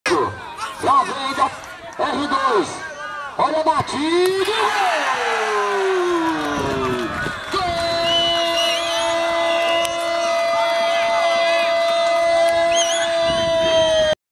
GOL DO ATACANTE ELSON R-2 DEBONY
TERCEIRA RODADA DA FASE DE GRUPOS DA SUPER COPA PIONEER 2017 JOGO ENTRE AS EQUIPES DO R-2 DEBONY CONTRA OS DA POLTRONAS PARTIDA NA ARENA SANTA AMÉLIA (18/02/2017)